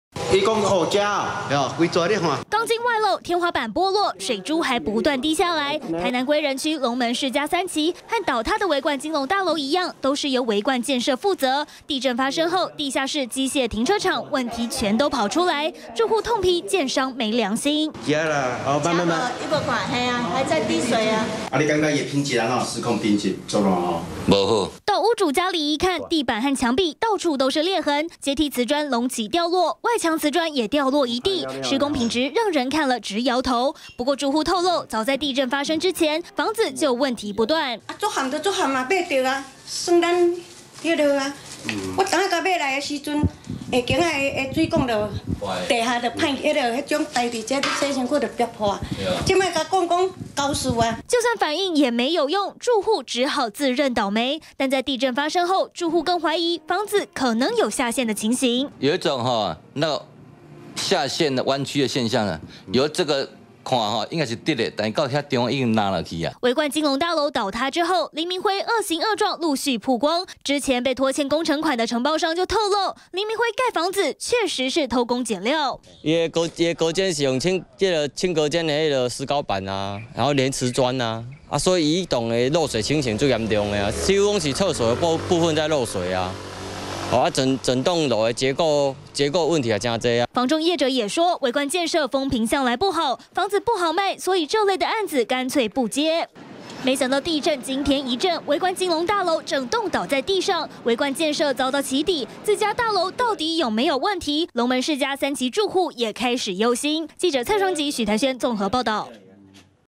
20160213中天新聞 維冠建設遭起底「龍門世家」3期震後也裂
地震導致維冠金龍大樓橫躺在地，維冠建設遭到起底，更有住戶爆料，維冠建設房屋問題多，中天新聞記者，就獨家直擊台南歸仁區的「龍門世家3期」，發現地震之後，地下停車場天花板剝落、鋼筋外露，還不斷滴水。住戶還說，其實在地震之前就問題不斷，痛批建商沒良心。